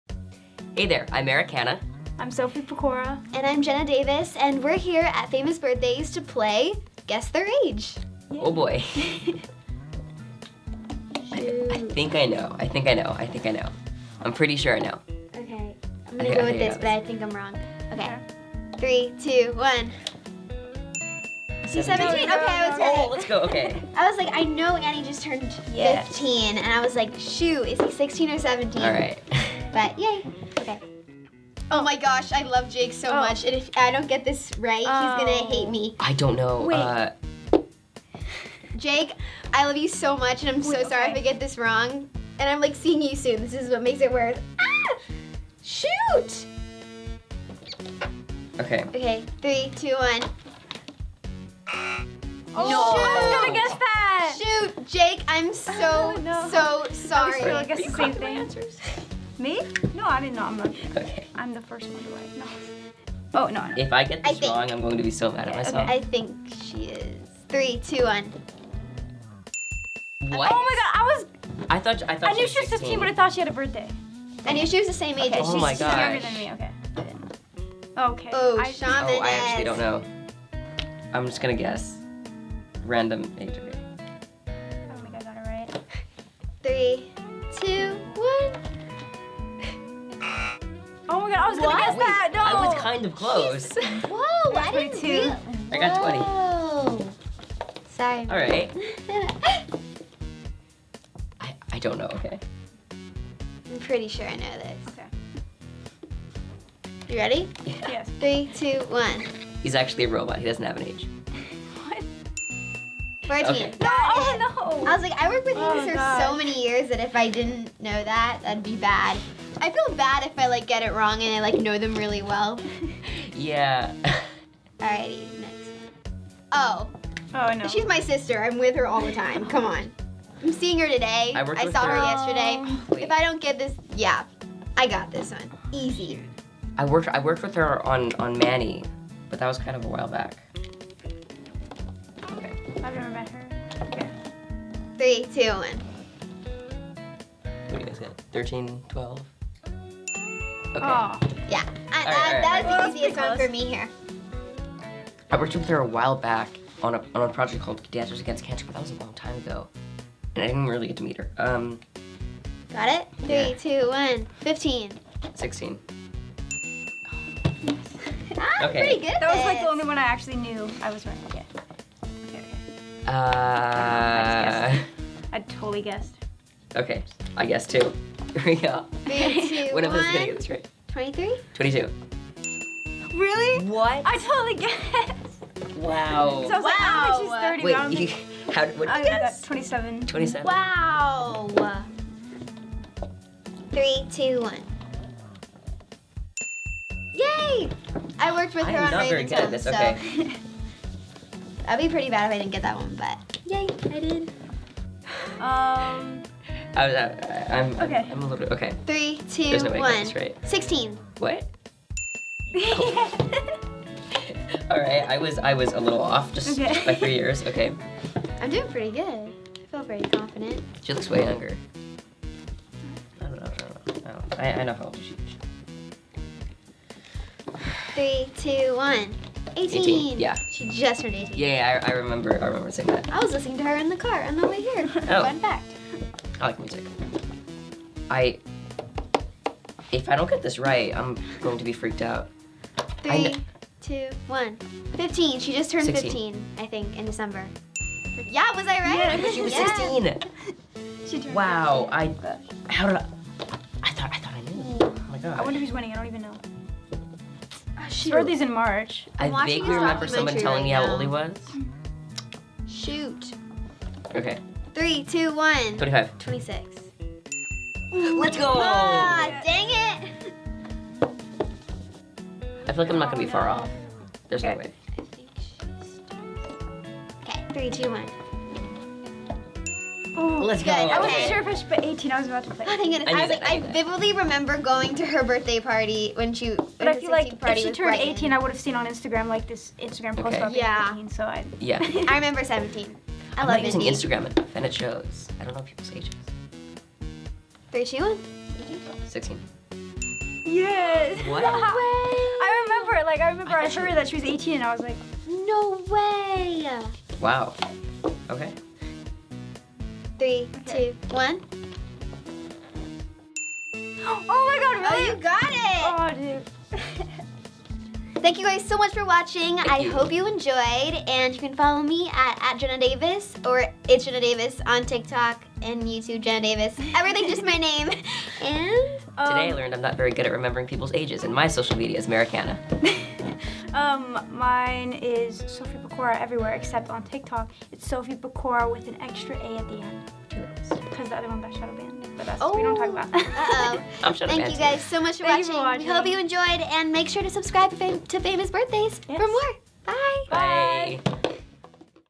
Sophie Pecora, Jenna Davis & Merrick Hanna - Guess Their Age
Sophie, Jenna and Merrick played Guess Their Age at Famous Birthdays on February 5th, 2020